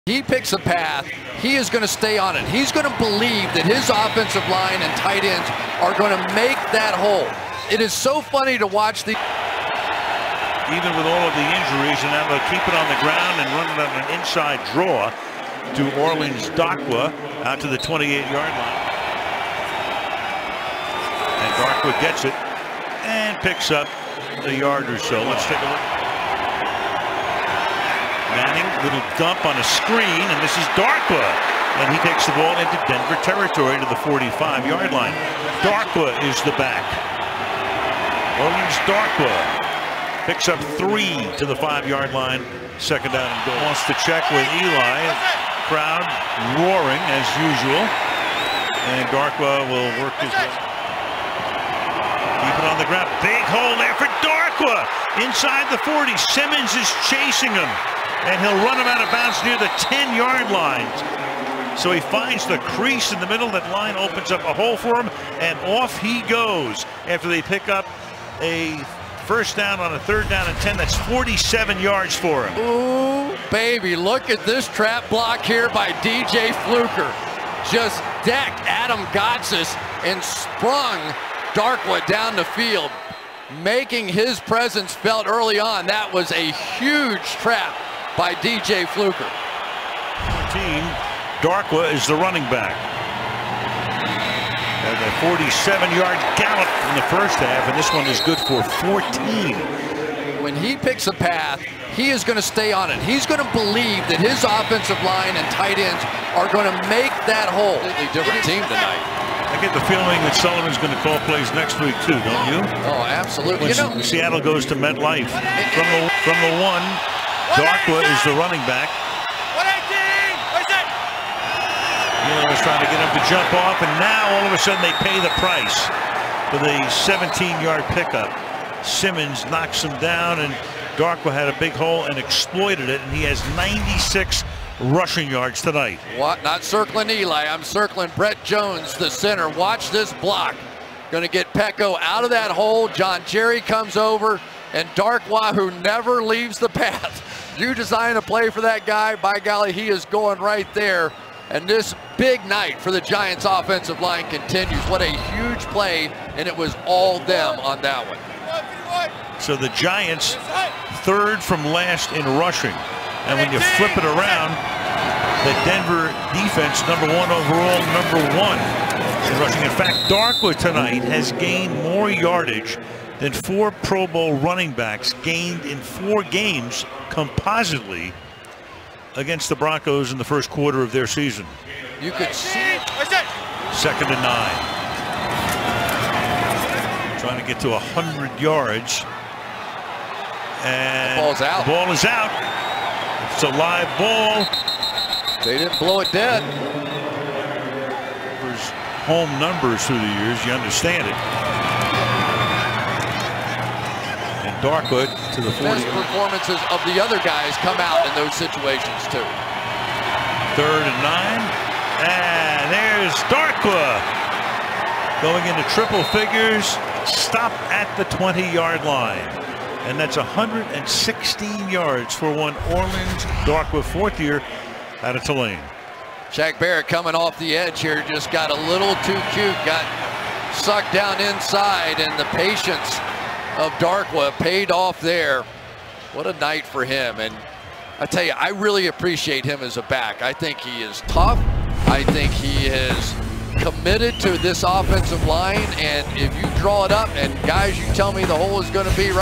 Orleans Darkwa Week 6 Regular Season Highlights Running Hard! | 10/15/2017
Orleans Darkwa Regular Season Week 6 Full Highlights 21 Carries 117 Yards 1 Catch 13 Yards